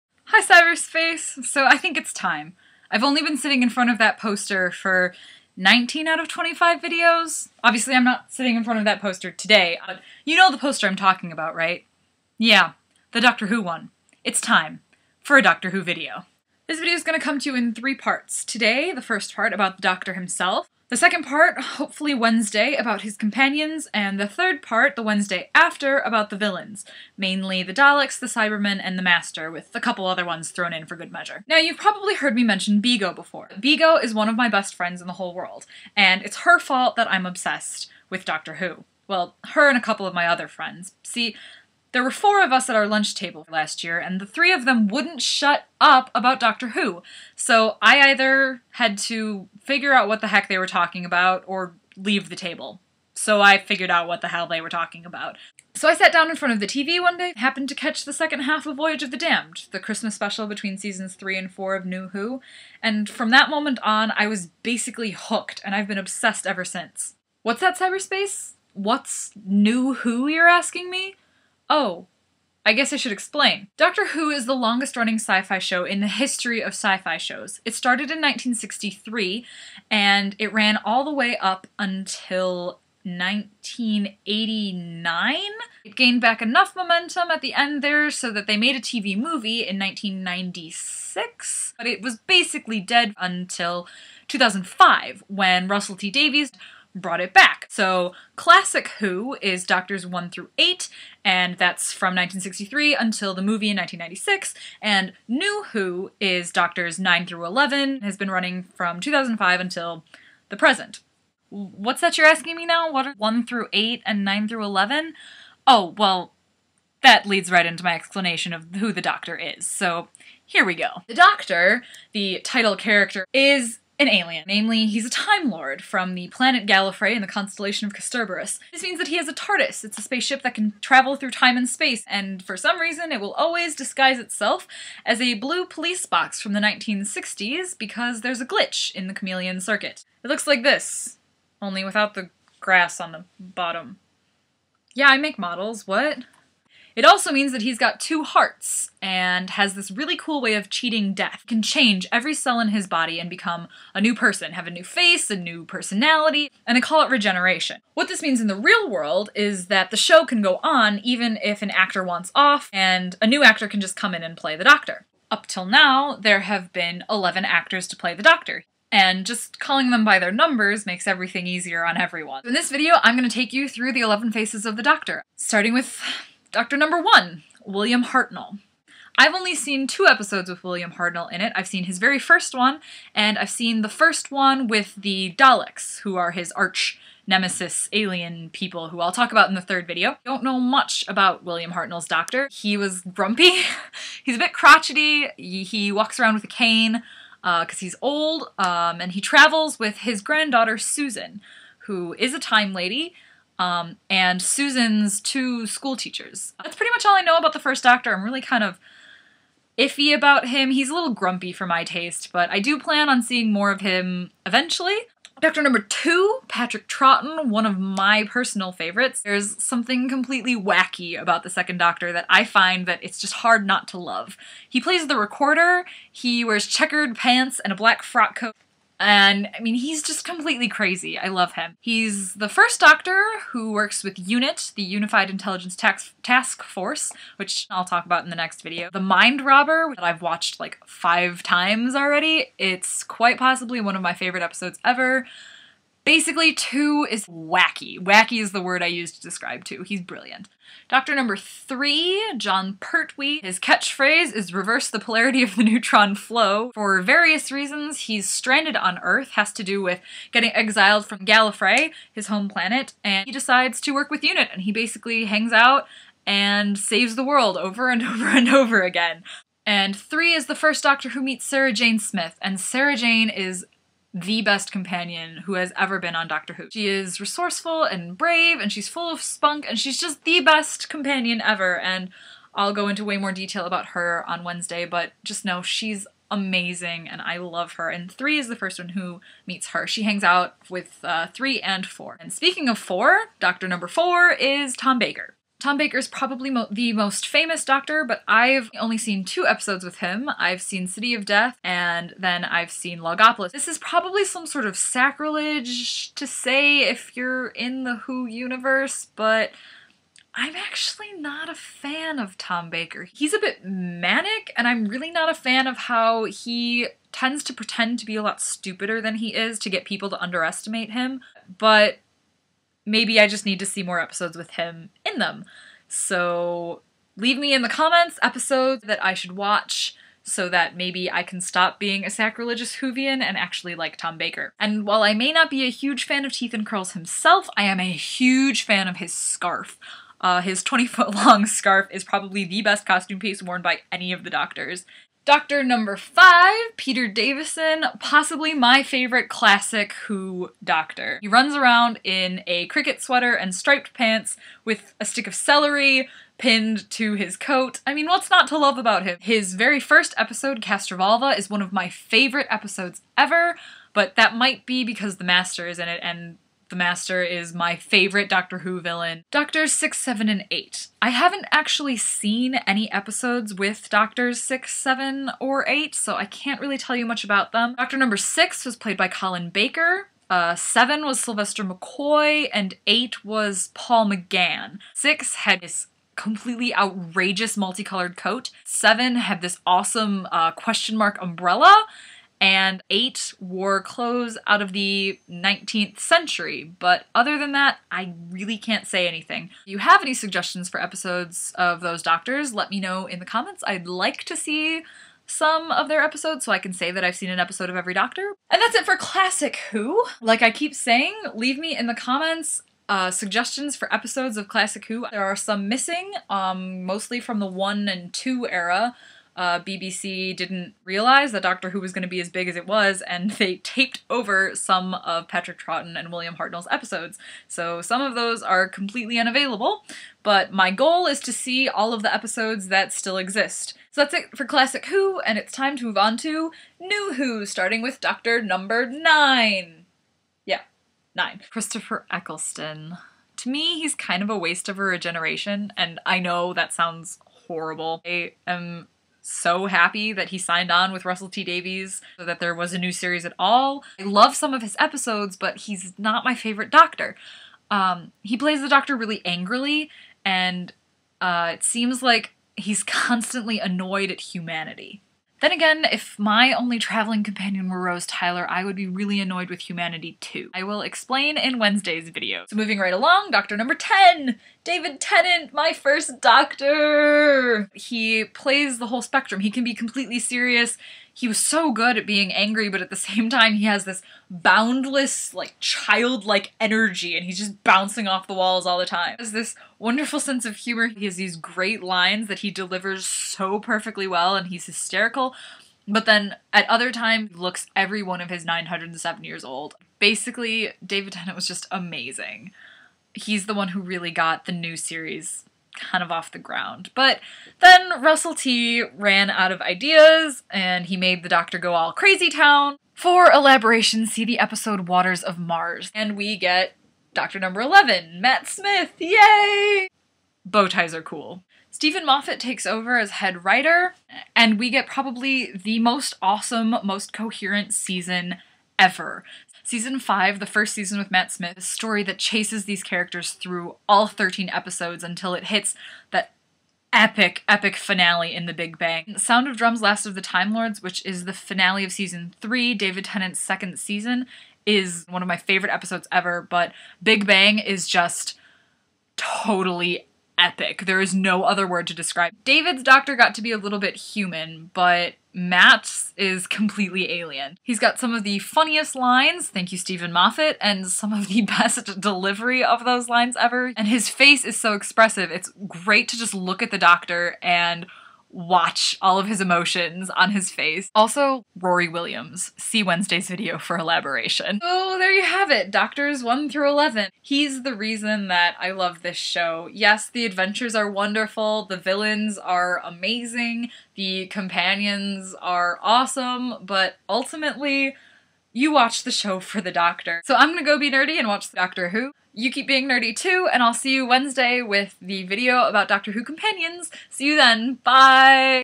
Vlog15a: Doctor Who (no spoilers)
PART ONE: THE DOCTOR: in which Megan finally talks about Doctor Who. NOTE: this is NOT a reaction video to the new season! It's a vlog that comes in three parts, starting with--of course--the 11 faces of the Doctor himself!